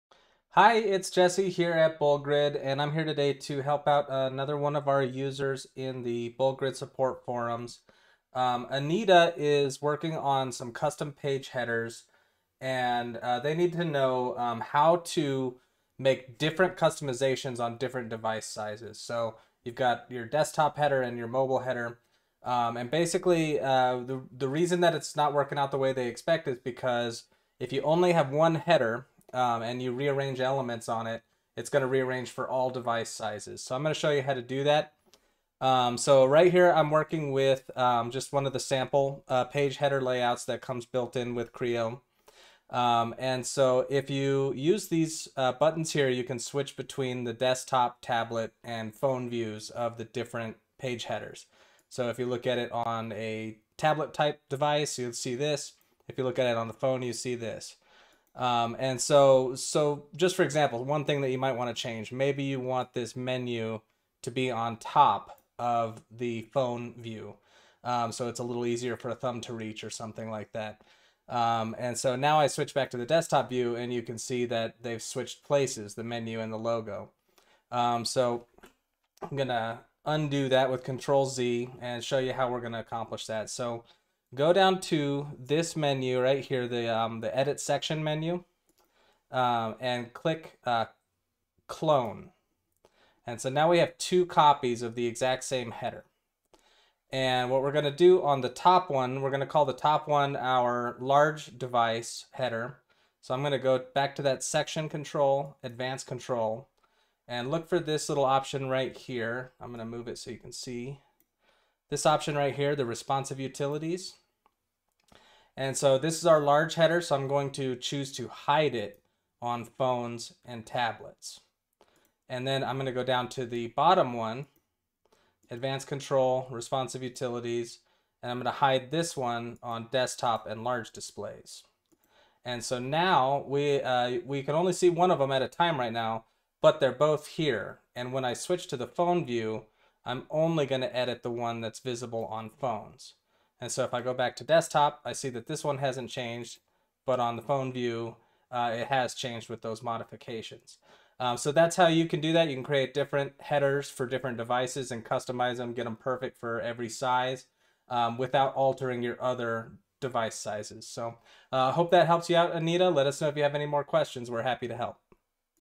How to Customize Headers for Mobile Devices in WordPress
Using Crio's new Custom Page Headers feature, you can customize your website's header to look great on a Desktop, Tablet or Phone. Full instructions
Jesse walks you through how to use the mobile responsive utilities to create a different header for each device size.